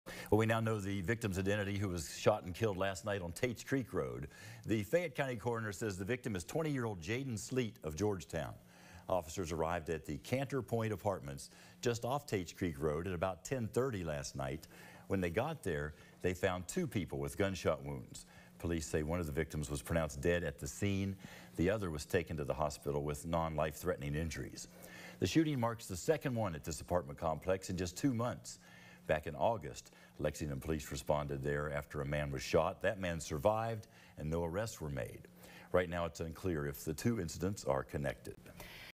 20-year-old dead, 1 hospitalized in shooting near Tates Creek Road
The Fayette County Coroner's Office has released the name of the individual who died during the shooting Thursday night.